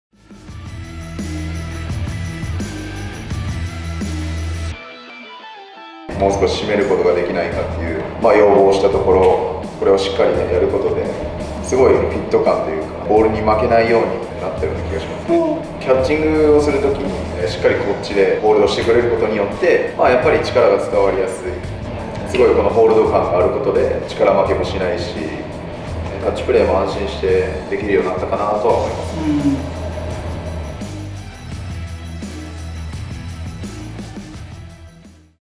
【SSK野球公式】阪神タイガース梅野選手の声から生まれた、SSKキャッチャーミット新機能「U-LOOP」
阪神タイガース梅野選手の声から生まれた新機能。

20秋冬シーズンではproedge硬式・軟式の梅野選手モデルのキャッチャーミットに搭載。

「ミットは使っていくにつれて薬指側が緩くなったり、反ってくるのが気になってたんです。ユーループは、引っ張ってその都度調節できるので、ボールに負けないフィット感がありますね。体から離れたボールを捕るときもしっかり指先に力が伝わるし、タッチプレーも安心してできるようになりました。」

■プロエッジオーダーグラブシミュレーション

■スマホでデザイン！SSK昇華ウエアシリーズ